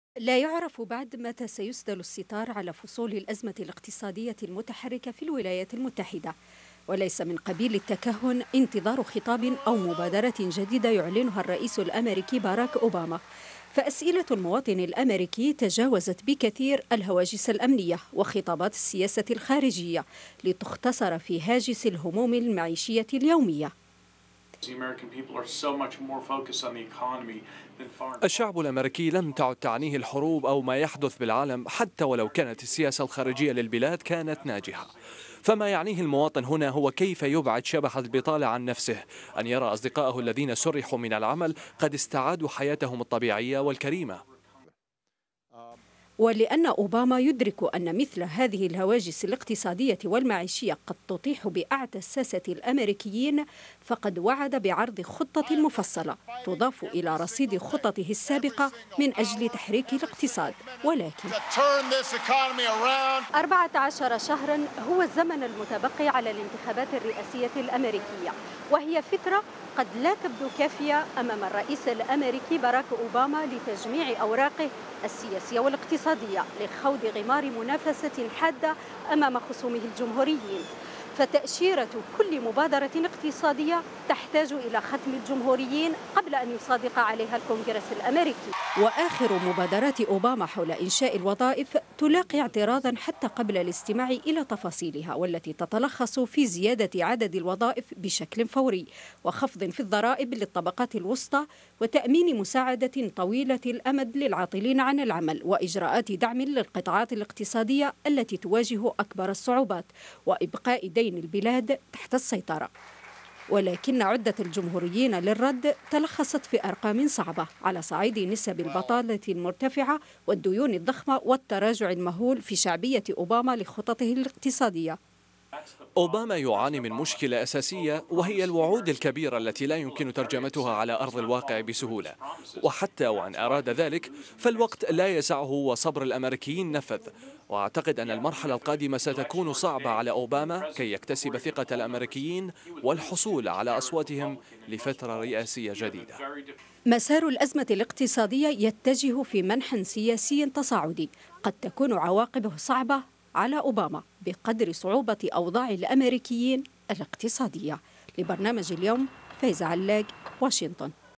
هل ستنجح خطة أوباما لتحريك الاقتصاد الأميركي الراكد؟
طالب الرئيس الأمريكي باراك أوباما الكونجرس باعتماد حزمة وظائف بقيمة 450 مليار دولار، وذلك في آخر خطاب له في اطار مقترحاته الرامية الى تعزيز فرص العمل واقالة الاقتصاد من عثرته.
وجاء في صدارة تلك التدابير مقترحا لتخفيض ضريبة الرواتب التي تصل نسبتها الى %6.2 والتي تمول المعاشات التقاعدية الحكومية الى النصف بشكل مؤقت.
ويتضمن مقترح أوباما أيضا تخصيص 50 مليار دولار للانفاق العاجل على الطرق السريعة والسكك الحديدية ومشروعات تجارة التجزئة والطيران، علاوة على مبالغ اضافية لتحديث مباني المدارس العامة.